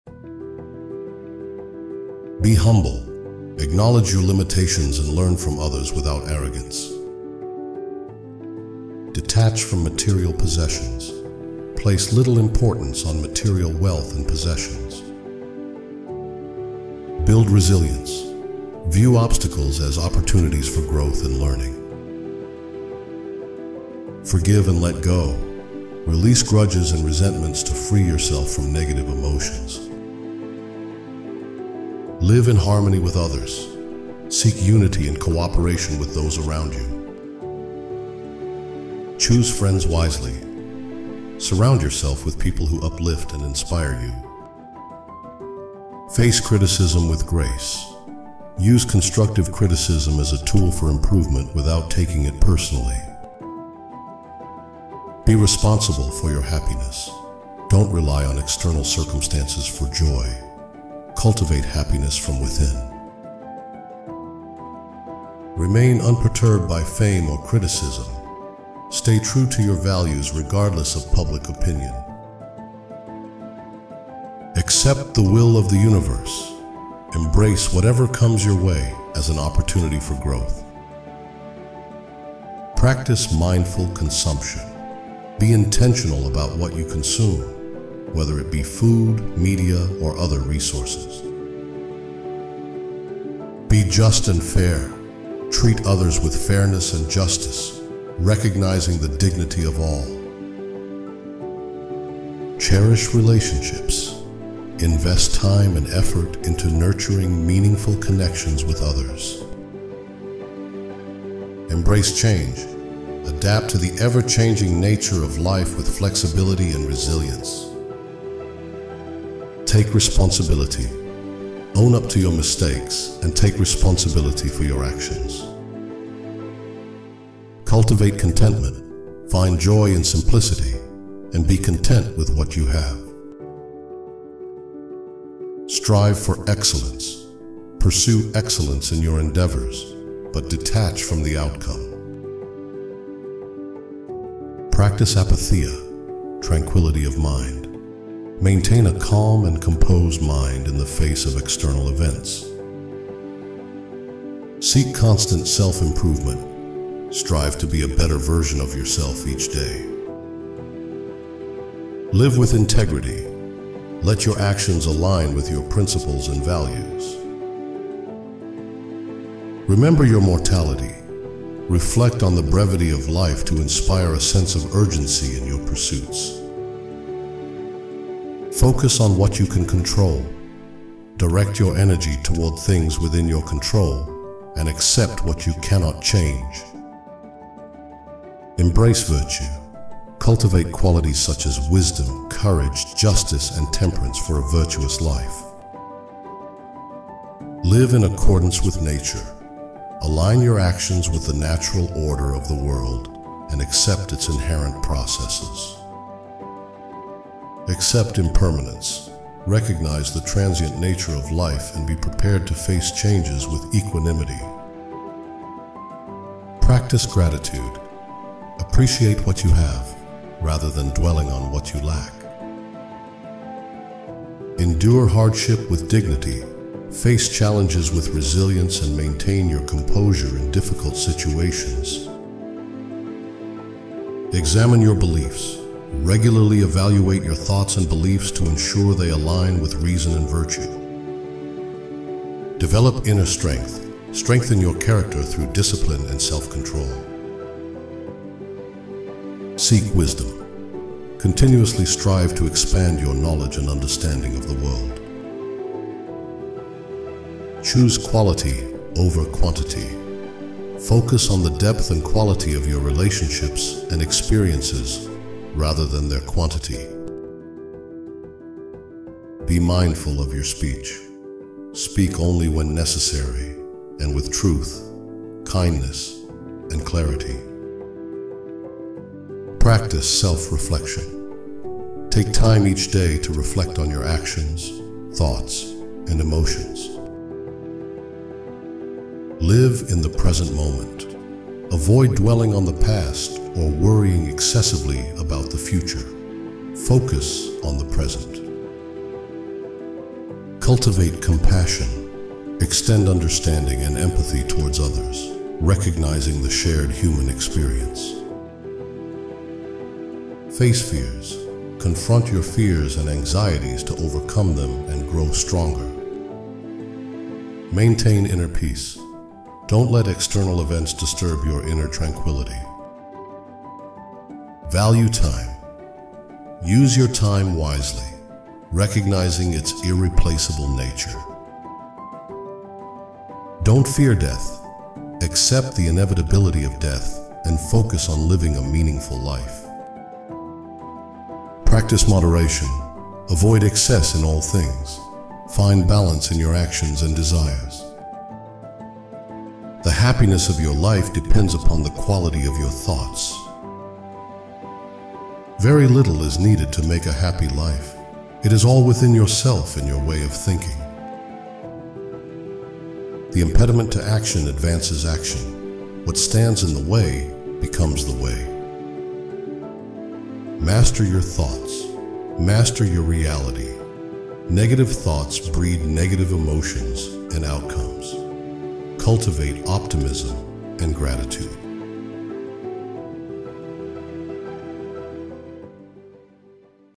The Stoic Guide to Conquering 2024: Stress Less, Achieve More Everyday Life Stoic Rules
2024 Vision: Navigate life's waves with ancient wisdom
Imagine a year where challenges transform into opportunities, setbacks become stepping stones, and happiness isn't fleeting, but anchored in something deeper. This is the promise of Stoicism, an ancient philosophy that's surprisingly relevant for modern life.

In this topic, we'll explore Stoic rules that aren't just trendy resolutions, but timeless principles for a more resilient, fulfilling life in 2024 and beyond.